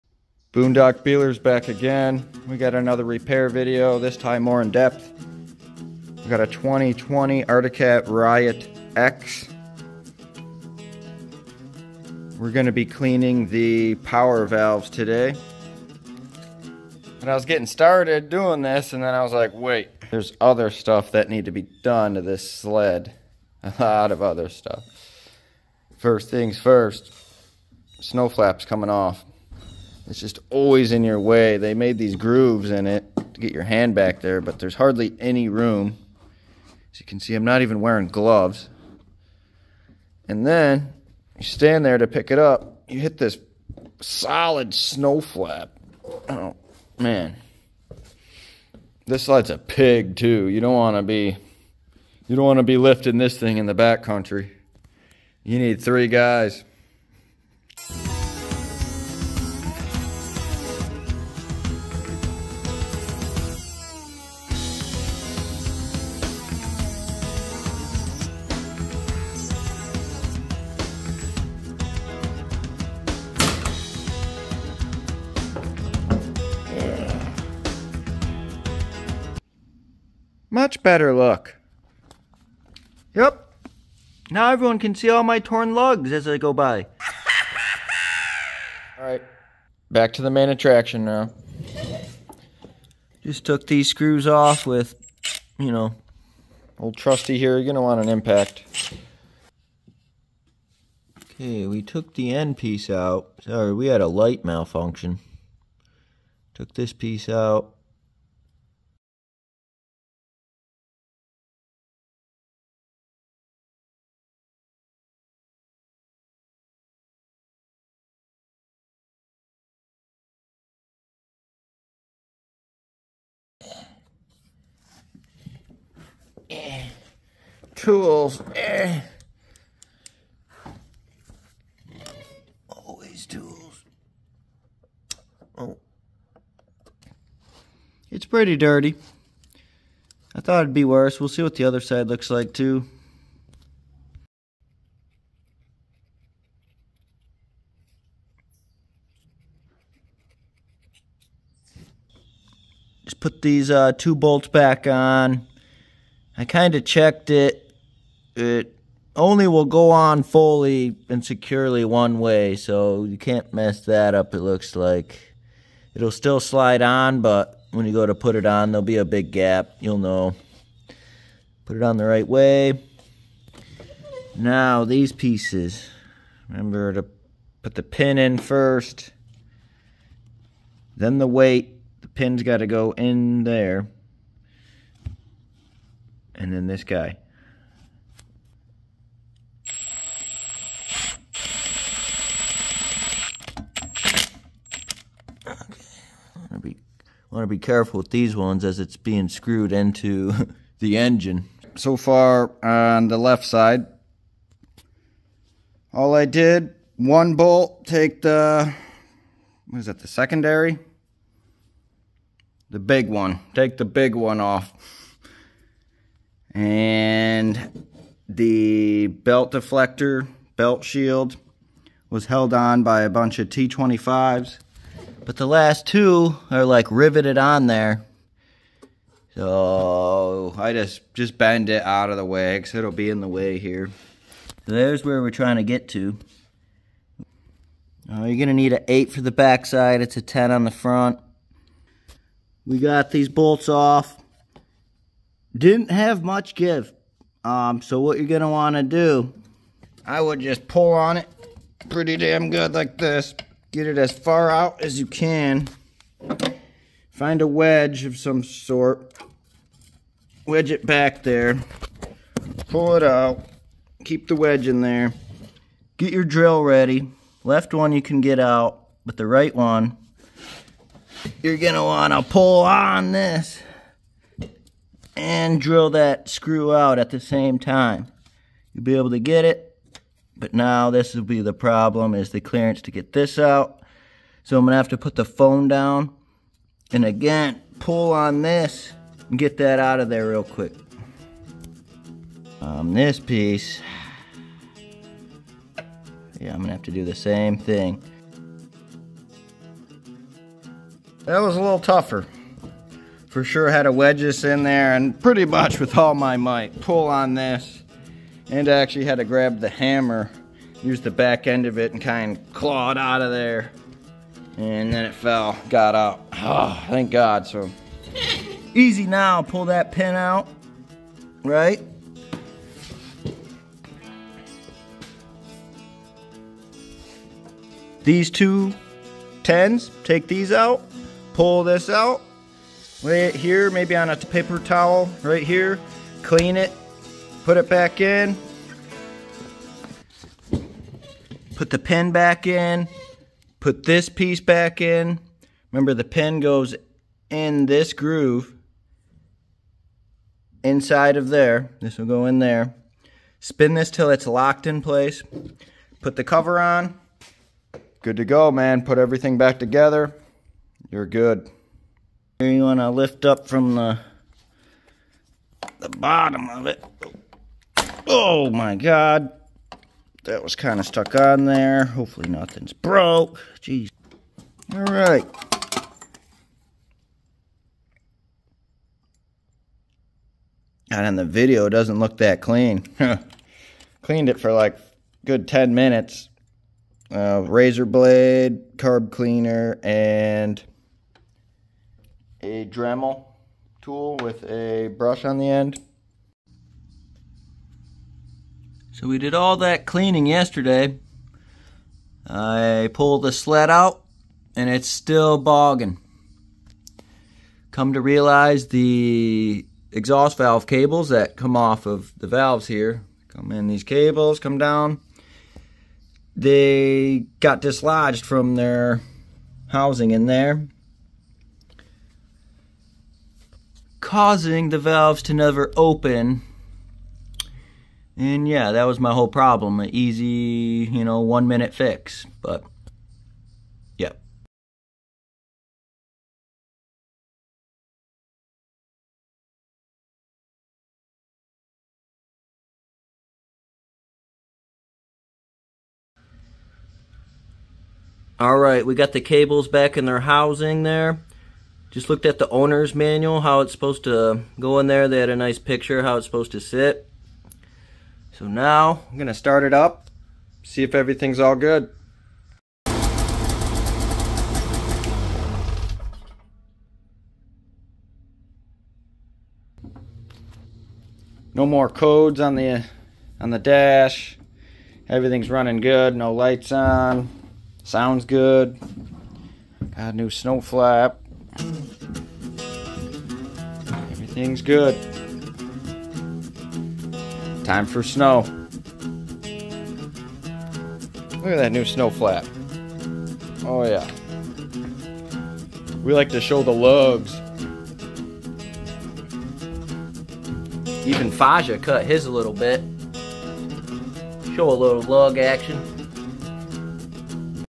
Arctic Cat Riot X Power Valves Cleaning and Cable Re-Connect DIY. Problem Solved!
In this DIY tutorial, I will show you the process of cleaning the power valves and reconnecting the cables on a 2020 Arctic Cat Riot X snowmobile. If you're experiencing issues with power delivery, heavy bogging, poor throttle response, or performance problems, this guide will help you solve those issues and get your snowmobile running like it should!

I will show you how i safely disassemble, clean, and reassemble the power valves, along with tips on reconnecting the cables properly to ensure smooth operation. Whether you're a seasoned rider or a beginner, this guide will help you maintain your snowmobile for optimal performance.

0:00 Intro/Snow Flap Removal
1:30 Power Valve Removal/Cleaning
6:40 Cable Re-connect Issue Found